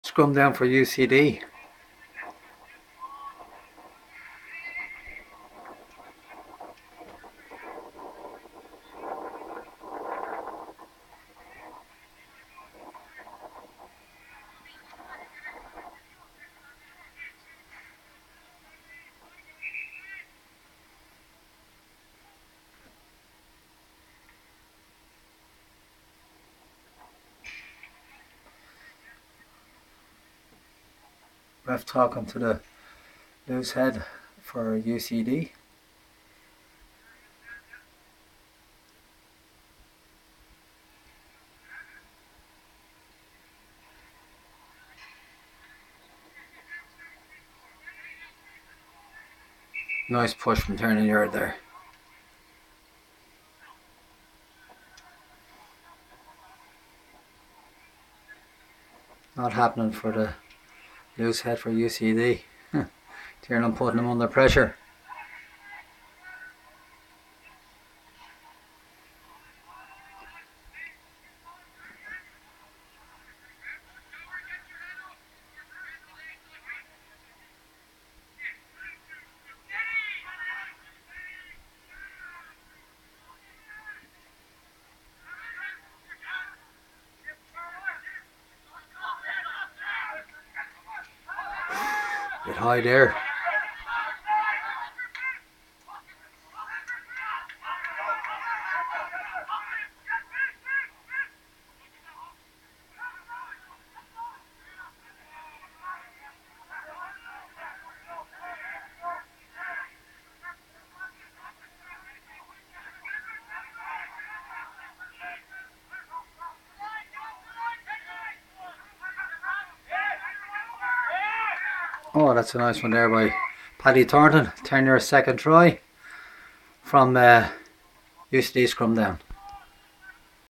UCD PACK UNDER PRESSURE TIARNÁN CREAGH TIGHT HEAD IN CONTROL OF UCDs LOSE HEAD.
Tiarnán Creagh tight head for Terenure is in control of UCDs lose head in every scrum, this clip shows UCD under pressure from scrum to Try,scored by Paddy Thornton Terenure Flanker, you can see Tiarnán play along the way to the Try.